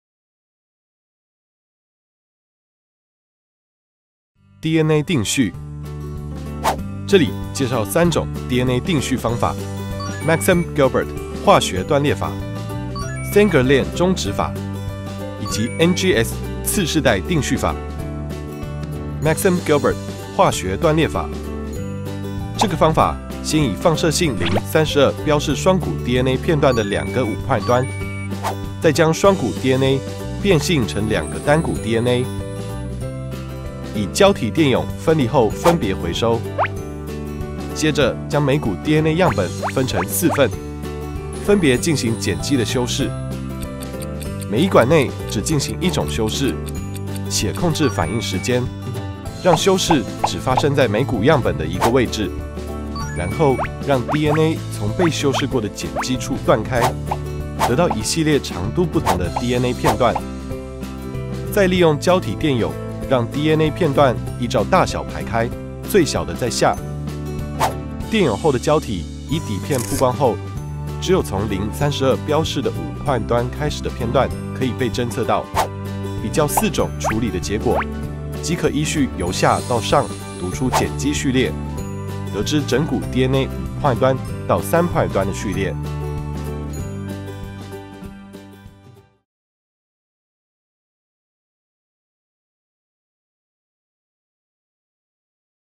(5-3) 定序-化學斷裂法_原理 (動畫)
臺大開放式課程
課程名稱：生化實驗技術：原理 (動畫) 與操作示範
授課教師：生物技術研究中心 生化實驗教學資源工作室 王惠鈞 等